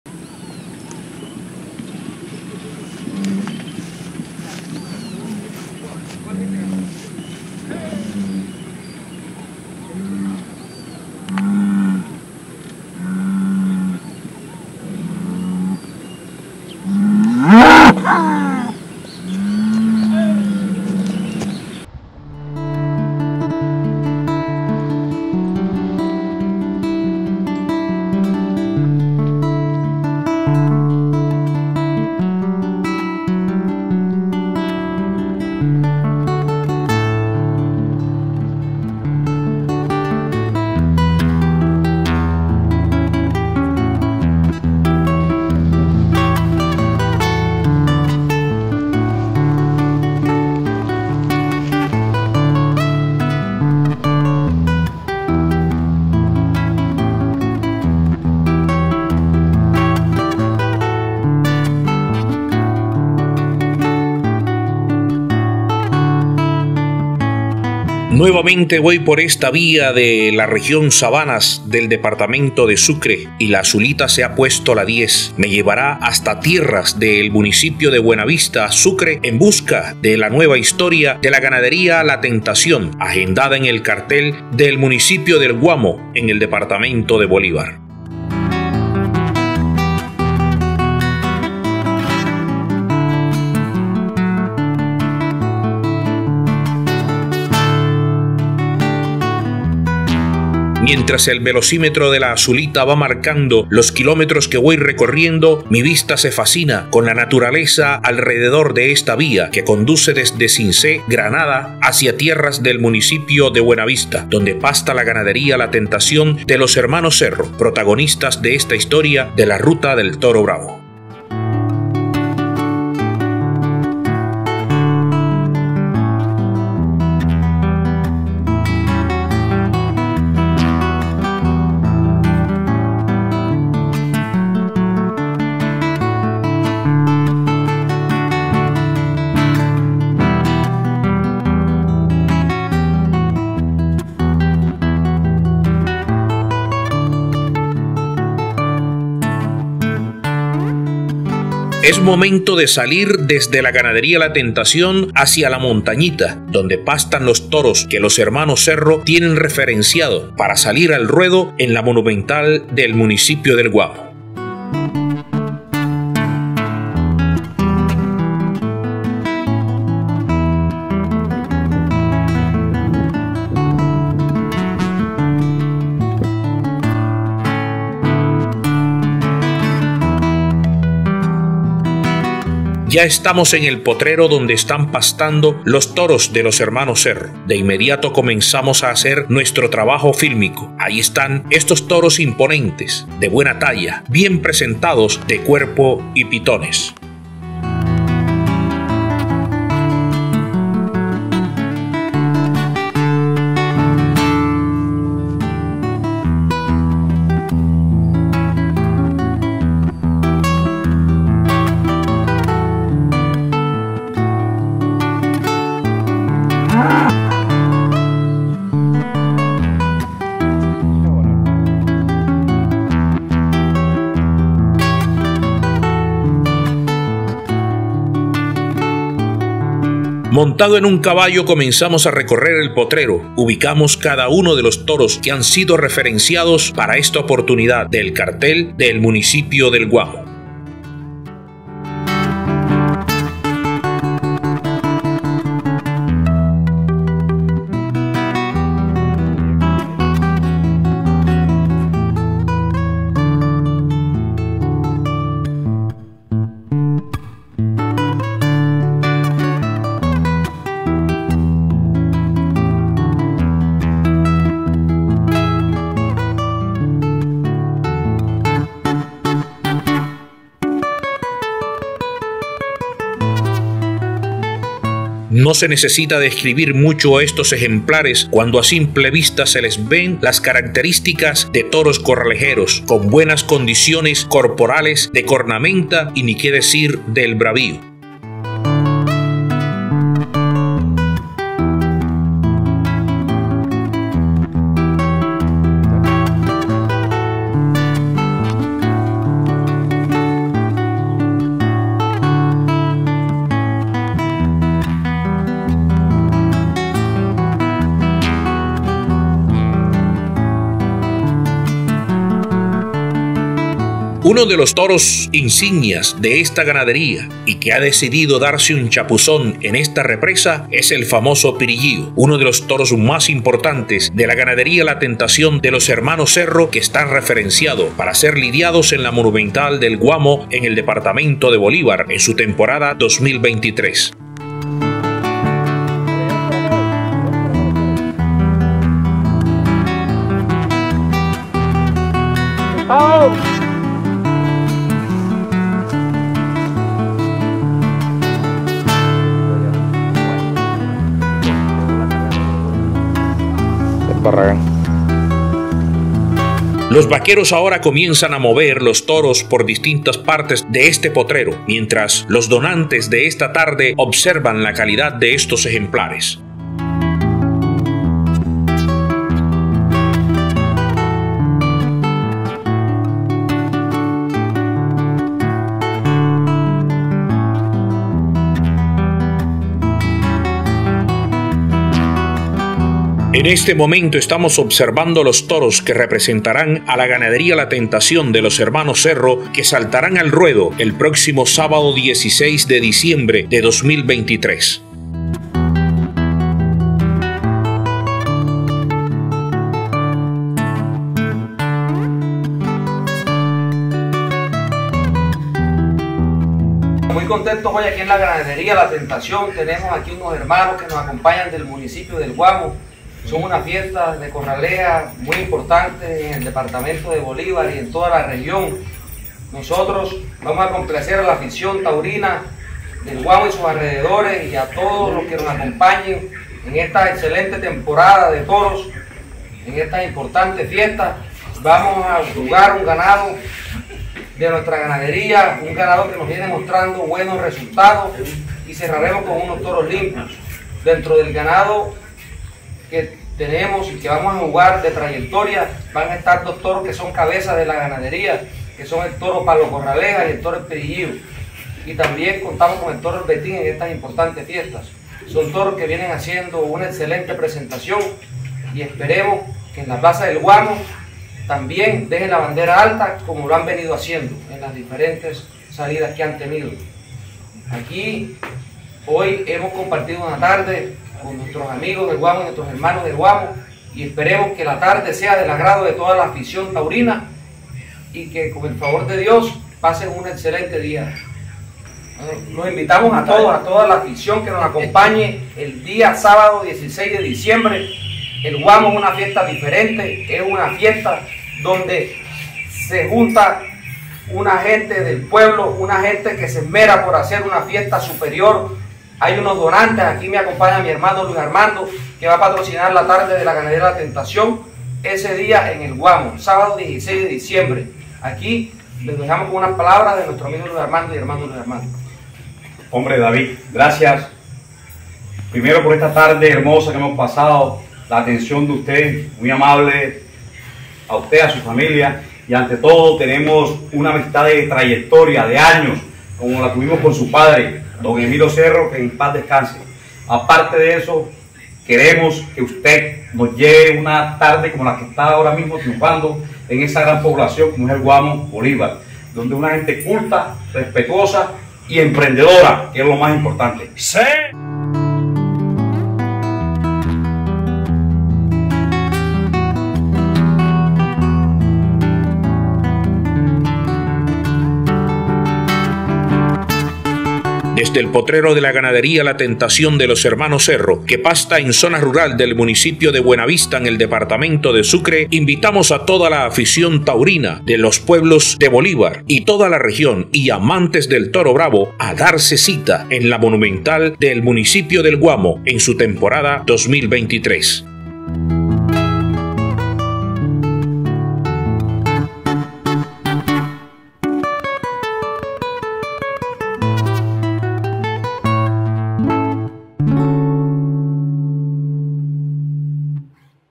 GANADERÍA LA TENTACIÓN PARA EL GUAMO - BOLÍVAR 2023
Estos son los toros de la ganadería La Tentación, que los hermanos Cerro seleccionaron para jugar en el municipio de El Guamo Bolivar 2023.